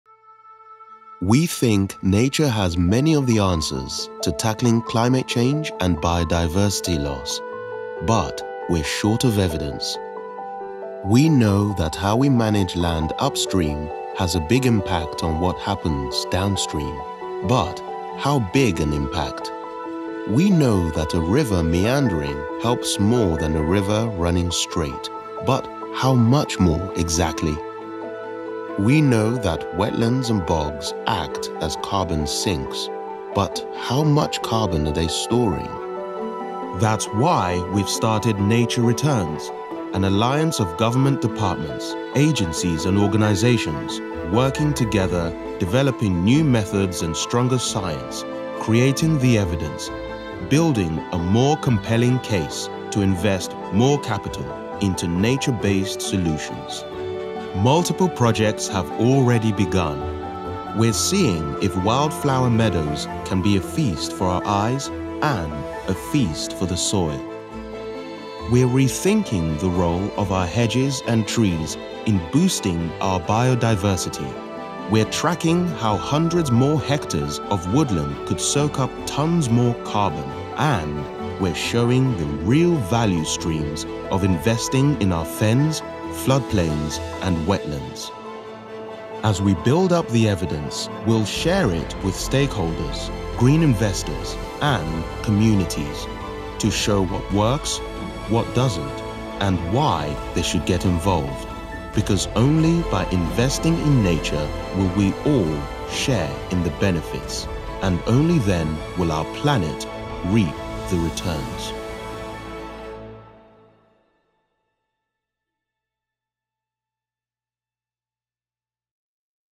Nature returns - testing Nature-based Solutions in practice
Nature has many of the answers to climate change and biodiversity loss. But to reap the returns, we need to invest in the research.

Nature Returns is a partnership of government departments, charities and organisations working together to build the evidence for nature-based solutions to climate change and biodiversity loss.

We back the science that asks what’s really happening inside peat bogs, grasslands and floodplains. Then we connect scientists, stakeholders and green investors to share what works, what doesn’t, and why. From carbon capture to sustaining biodiversity, we need evidence-based answers and community-based action. By investing in nature, we will all share in the benefits — and return the planet to a sustainable state.

To see our reports/ find out more and keep up to date with progress, please visit Nature Returns Programme